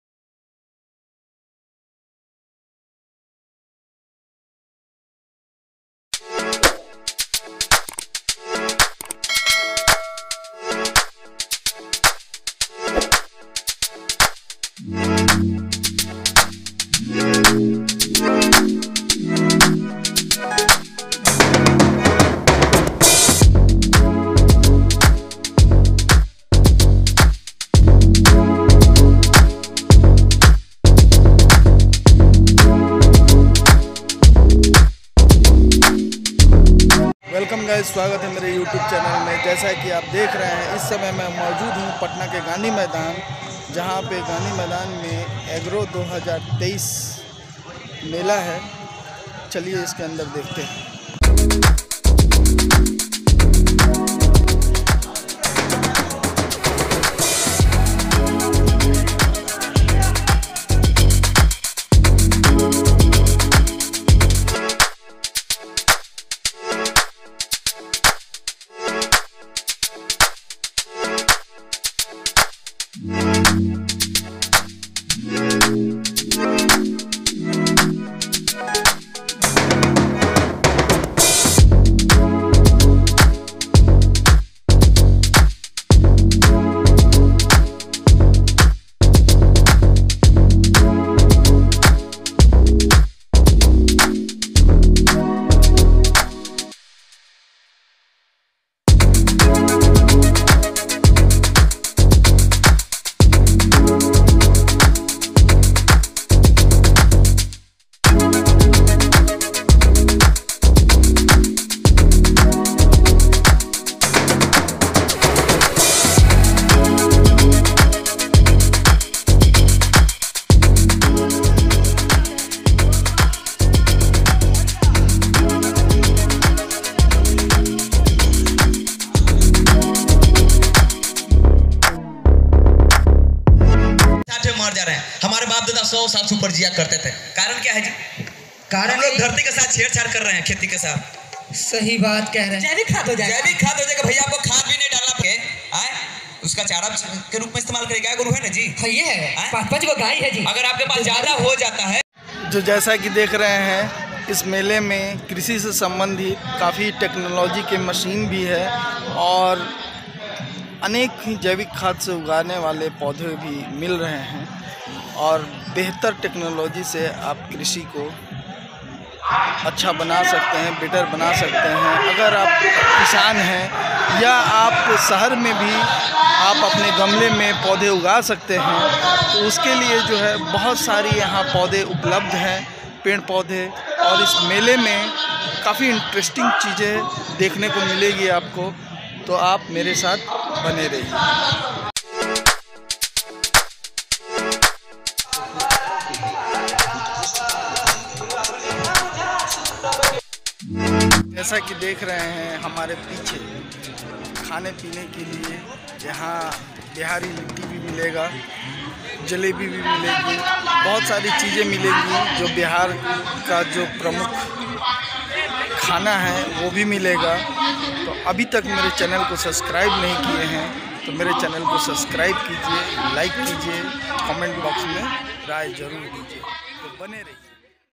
Agro 2023 Mela Patna Me || नई मशीनों ने बिहार के किसानों के लिए बेहतर तोहफा
ये विडियो पटना गांधी मैदान में बिहार एग्रो 2023मेला का है जिसमे कृषि से संबंधित सही प्रकार के माशिनो  जो खेती तथा समय में ज्यादा खेती तथा समय बचाया जा सकता है।जो लेबर संबंधित समस्या  मूल है।और किसानों को बोई कटाई था हार्बेस्टर ठकताई के साथ खेतो में प्लालाली ज्लानेवाली समस्याओं से निजात मिलेगी था सरकार द्वारा सब्सिडी  मिल रही है दो लाख की लागत वाली  मशीनों पर एक लाख तक सब्सिडी उपलब्ध है।